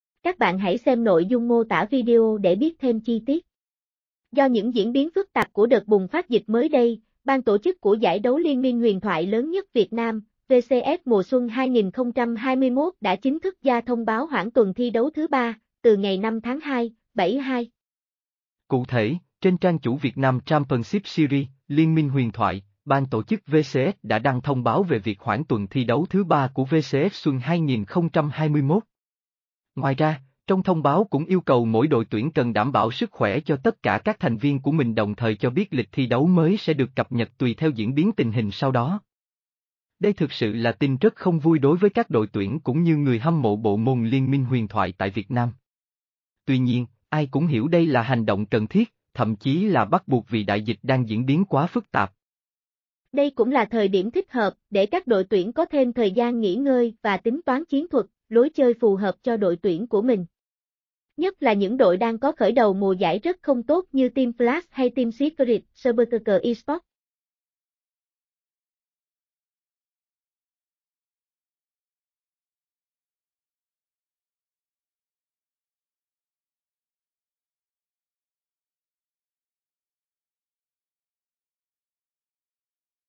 VCS mùa Xuân 2021 bị hoãn vì diễn biến phức tạp của dịch bệnh
Do những diễn biến phức tạp của đợt bùng phát dịch mới đây, BTC của giải đấu LMHT lớn nhất Việt Nam - VCS mùa Xuân 2021 đã chính thức ra thông báo hoãn tuần thi đấu thứ 3 (từ 5/2 - 7/2).
Cụ thể, trên trang chủ Vietnam Championship Series - LMHT, BTC VCS đã đăng thông báo về việc hoãn tuần thi đấu thứ 3 của VCS Xuân 2021. Ngoài ra, trong thông báo cũng yêu cầu mỗi đội tuyển cần đảm bảo sức khỏe cho tất cả các thành viên của mình đồng thời cho biết lịch thi đấu mới sẽ được cập nhật tùy theo diễn biến tình hình sau đó.
Đây thực sự là tin rất không vui đối với các đội tuyển cũng như người hâm mộ bộ môn LMHT tại Việt Nam. Tuy nhiên, ai cũng hiểu đây là hành động cần thiết, thậm chí là bắt buộc vì đại dịch đang diễn biến quá phức tạp.
Đây cũng là thời điểm thích hợp để các đội tuyển có thêm thời gian nghỉ ngơi và tính toán chiến thuật, lối chơi phù hợp cho đội tuyển của mình. Nhất là những đội đang có khởi đầu mùa giải rất không tốt như Team Flash hay Team Secret, SBTC Esports.